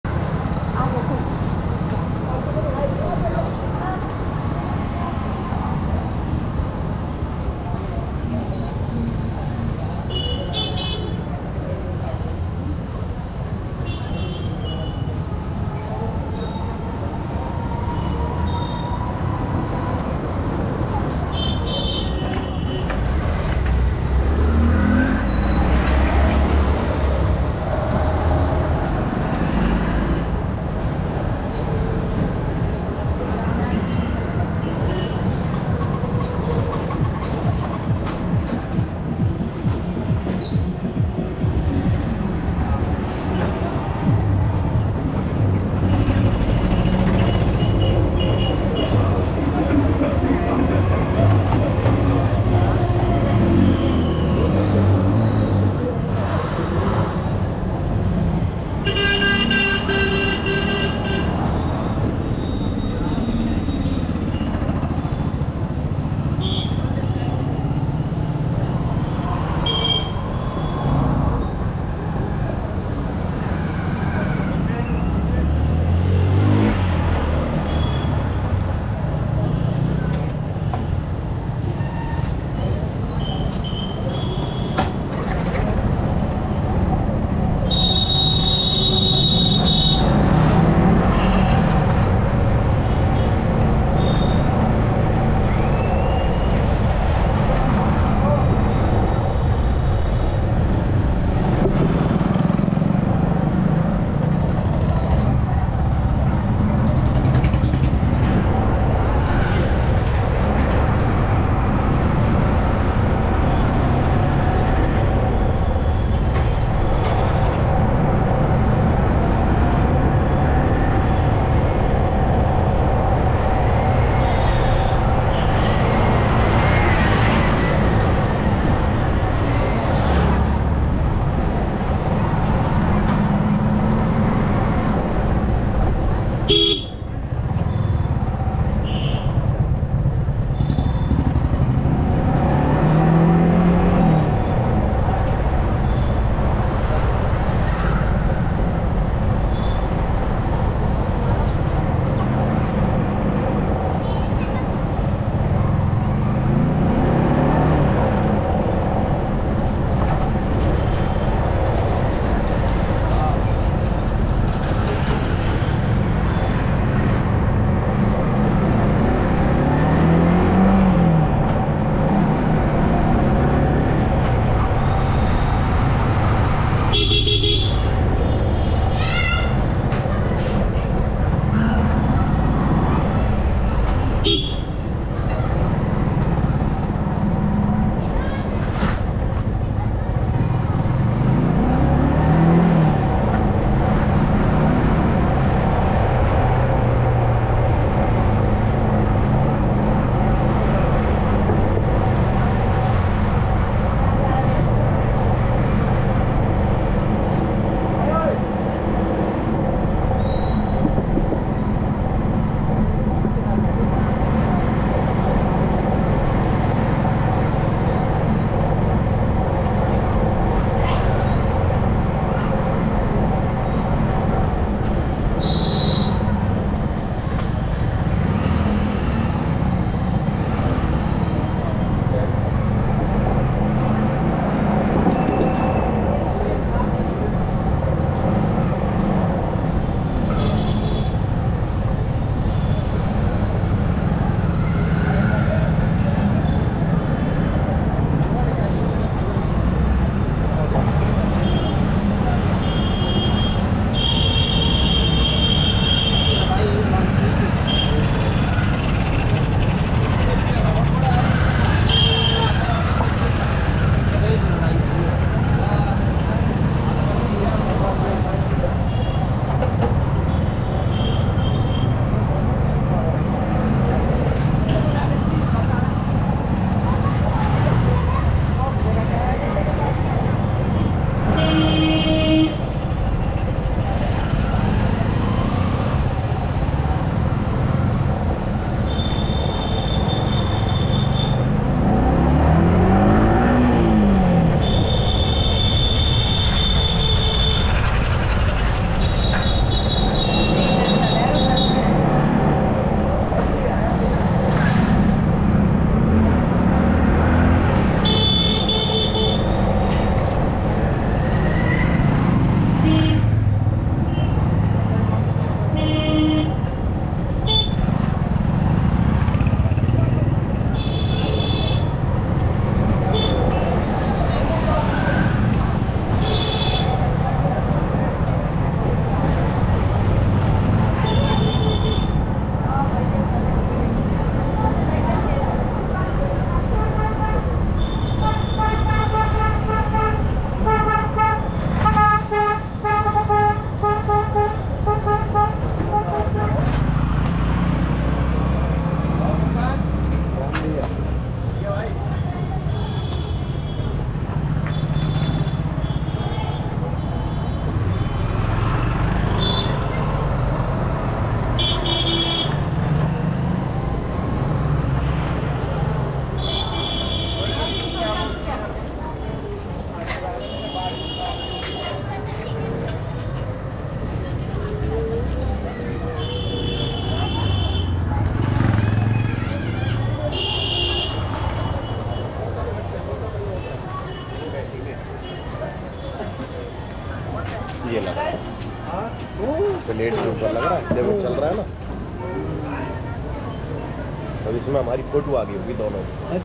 9 August 2023 |11| Bike Vlog | Jaitpur Gurudwara via Panchsheel Public School, 69 Bike Modification
See Video in High Quality